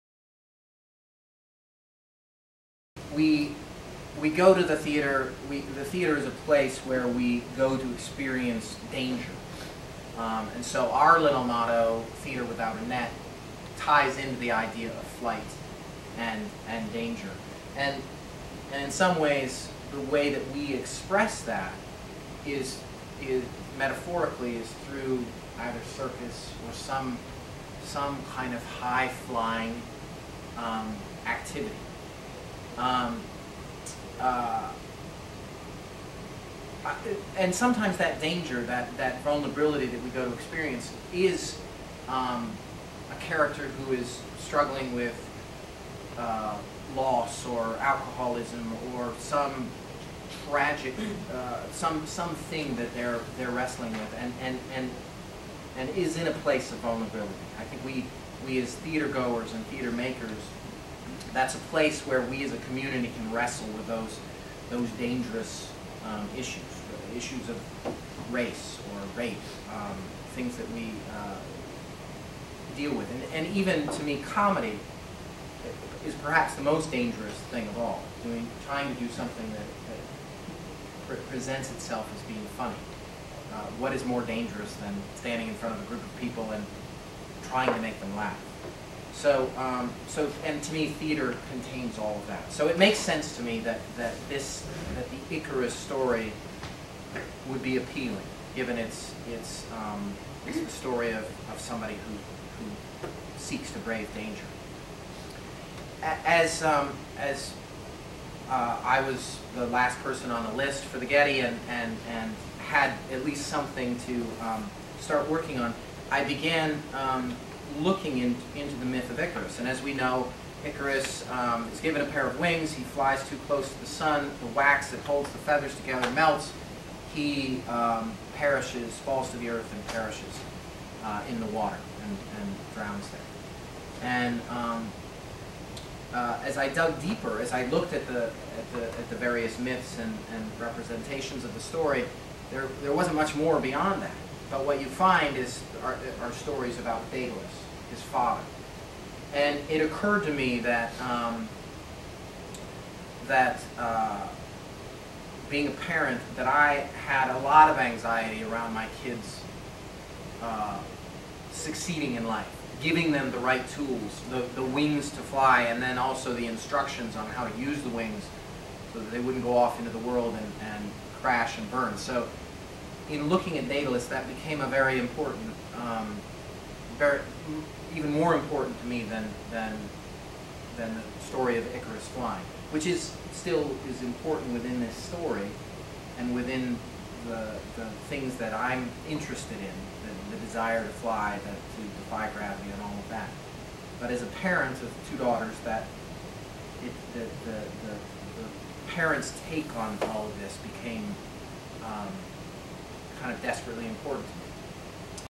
David Catlin on Icarus
Artistic Director, David Catlin, shares his thoughts on the upcoming production of Icarus at Looking Glass Theatre